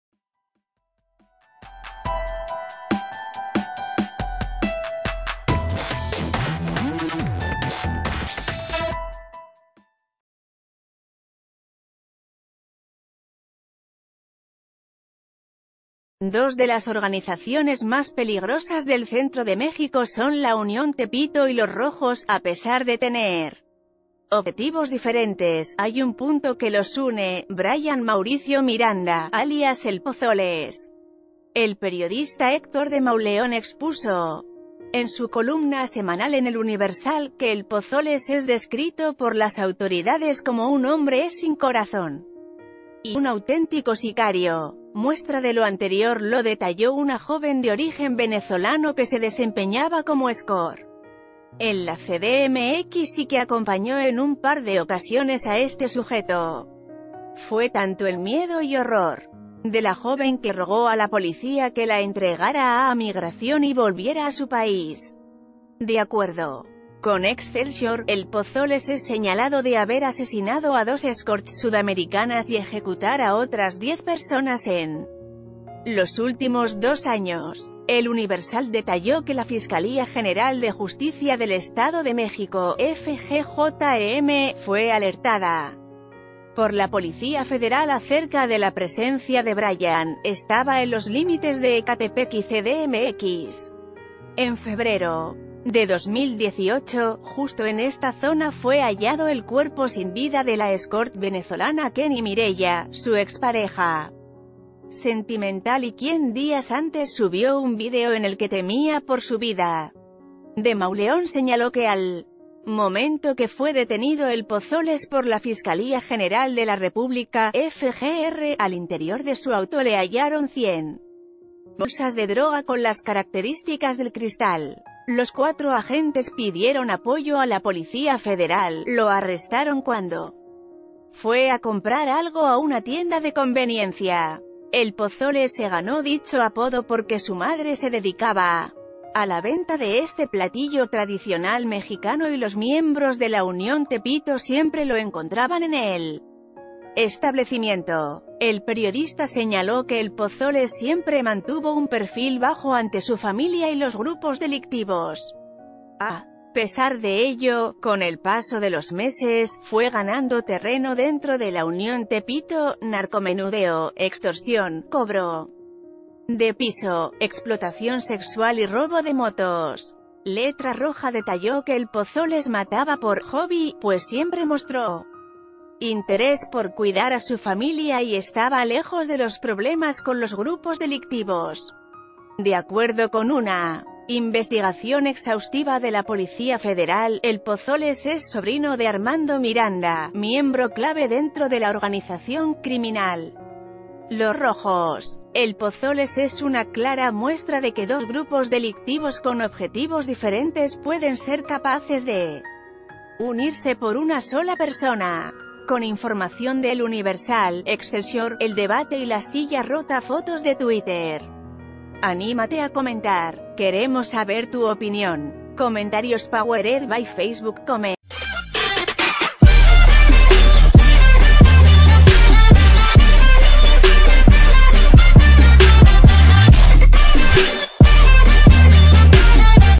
Brayan Mauricio: la relación de El Pozoles con el cártel de Los Rojos
Dos de las organizaciones más peligrosas del centro de México son La Unión Tepito y Los Rojos. A pesar de tener objetivos diferentes, hay un punto que los une: Brayan Mauricio Miranda, alias El Pozoles. El periodista Héctor de Mauleón expuso en su columna semanal en El Universal que El Pozoles es de...